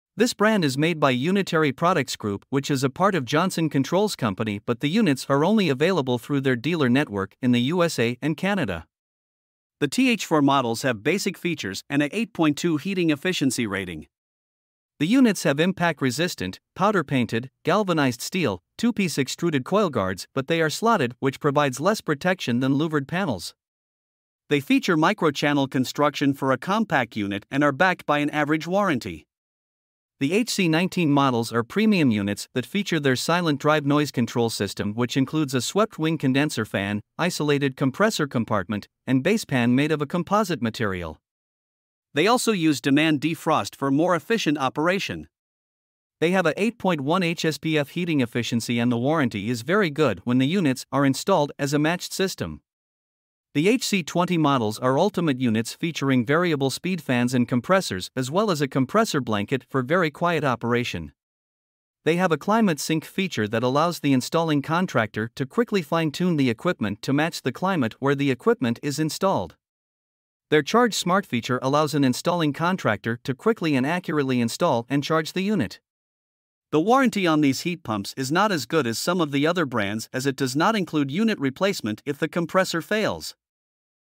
HvacRepairGuy 2023 Coleman Brand Heat Pump Reviews
In this video, HvacRepairGuy reviews the 2023 Coleman brand of heat pumps. The reviews include the basic TH14 models, the premium Echelon Series HC19 units, and the ultimate Echelon Series HC20 models.

You can get the reviews of all of the brands